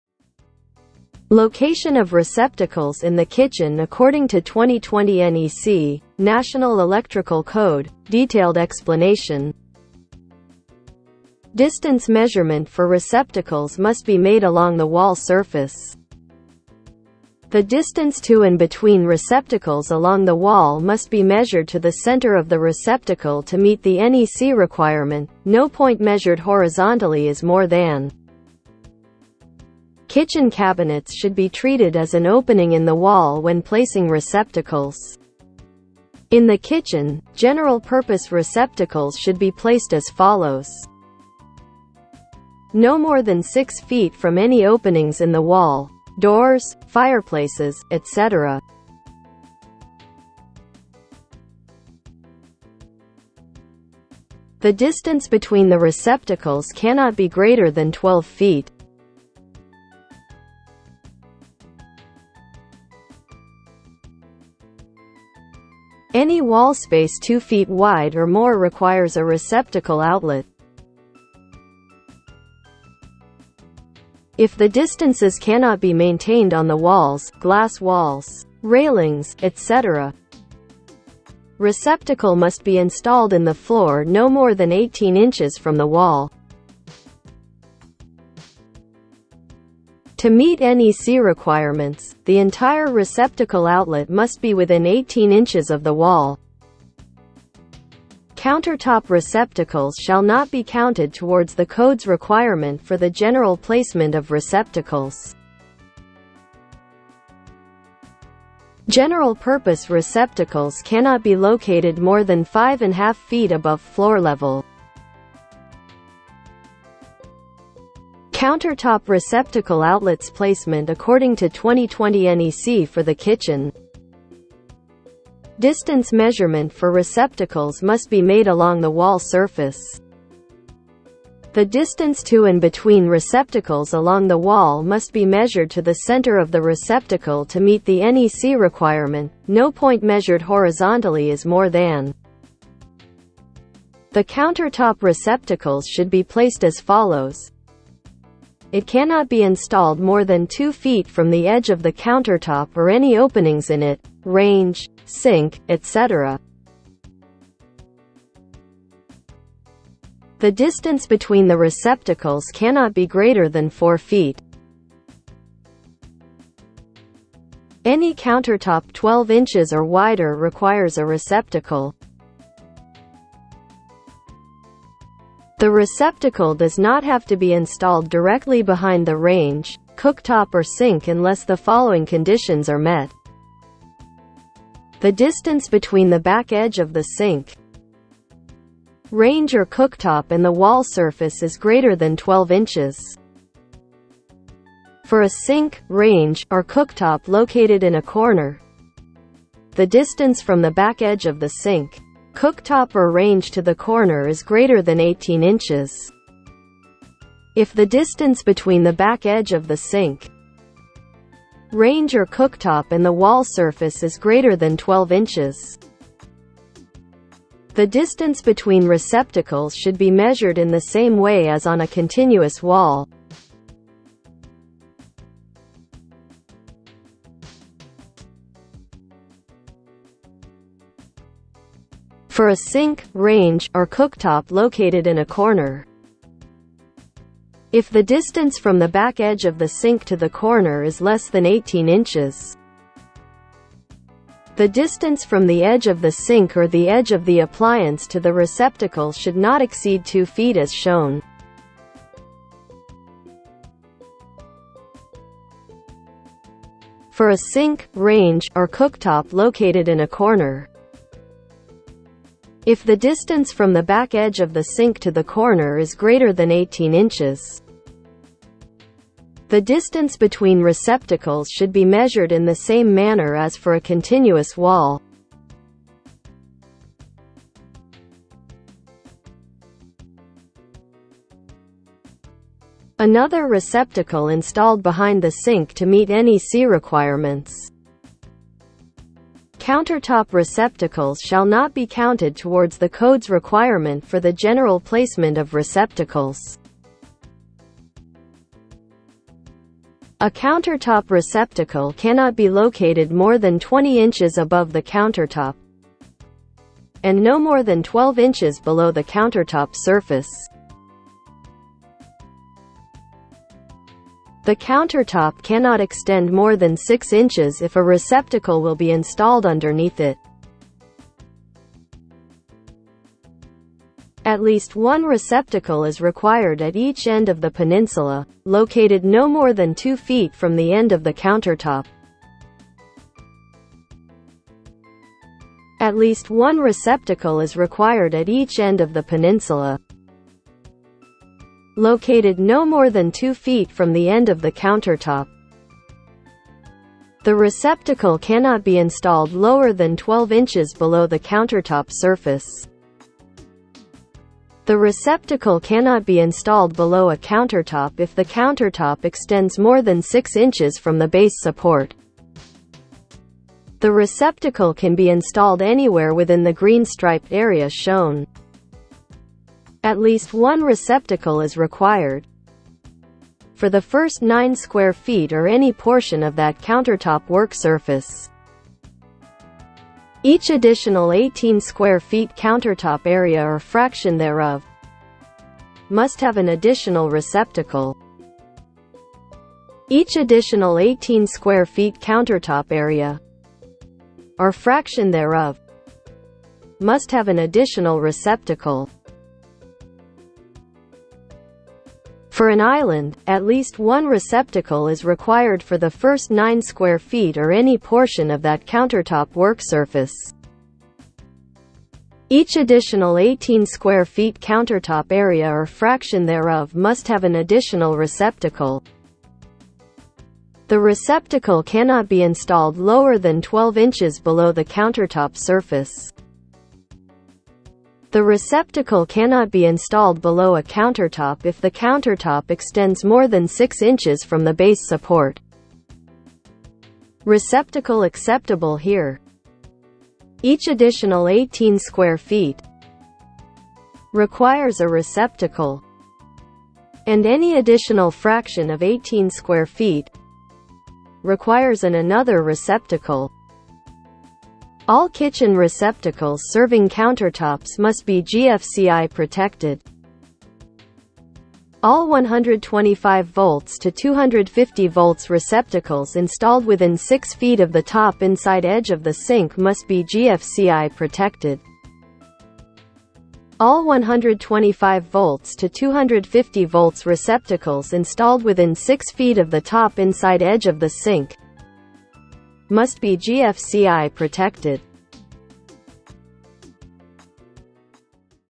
How to arrange receptacles in the kitchen in accordance with National Electrical Code.
Receptacle placement in kitchen under 2020 NEC.
Location of receptacles in the kitchen according to 2020 NEC (National Electrical Code) – detailed explanation.